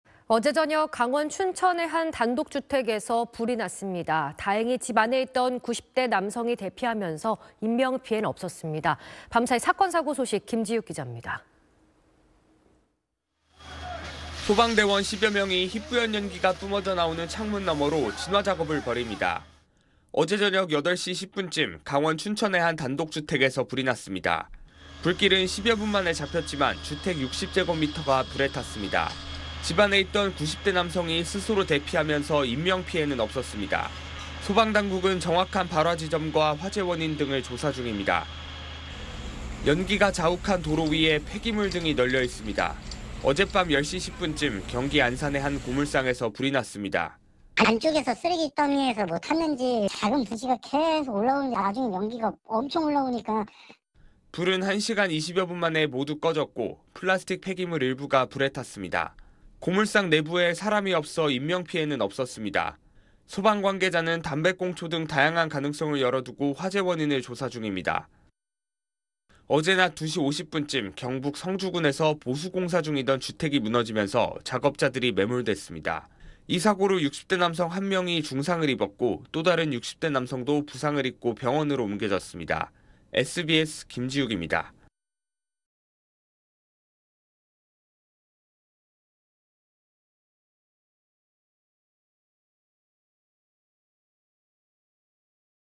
강원 춘천서 단독주택 화재…90대 남성 스스로 대피 / SBS
〈앵커〉

어제(25일) 저녁 강원 춘천의 한 단독주택에서 불이 났습니다. 다행히 집 안에 있던 90대 남성이 대피하면서, 인명피해는 없었습니다.

밤사이 사건사고 소식, 김지욱 기자입니다.

〈기자〉

소방대원 10여 명이 희뿌연 연기가 뿜어져 나오는 창문 너머로 진화 작업을 벌입니다.

어제저녁 8시 10분쯤 강원 춘천의 한 단독주택에서 불이 났습니다.

불길은 10여 분 만에 잡혔지만, 주택 60제곱미터가 불에 탔습니다.

집 안에 있던 90대 남성이 스스로 대피하면서 인명피해는 없었습니다.

소방당국은 정확한 발화 지점과 화재 원인 등을 조사 중입니다.

연기가 자욱한 도로 위에 폐기물 등이 널려 있습니다.

어젯밤 10시 10분쯤 경기 안산의 한 고물상에서 불이 났습니다.

[목격자 : 안쪽에서 쓰레기더미에서 뭐가 탔는지 작은 불씨가 계속 올라오는데 나중에 연기가 엄청 올라오니까….]

불은 1시간 20여 분 만에 모두 꺼졌고 플라스틱 폐기물 일부가 불에 탔습니다.

고물상 내부에 사람이 없어 인명피해는 없었습니다.

소방 관계자는 담배꽁초 등 다양한 가능성을 열어두고 화재 원인을 조사 중입니다.

어제 낮 2시 50분쯤 경북 성주군에서 보수 공사 중이던 주택이 무너지면서 작업자들이 매몰됐습니다.

이 사고로 60대 남성 1명이 중상을 입었고 또 다른 60대 남성도 부상을 입고 병원으로 옮겨졌습니다.

(영상편집 : 최혜란)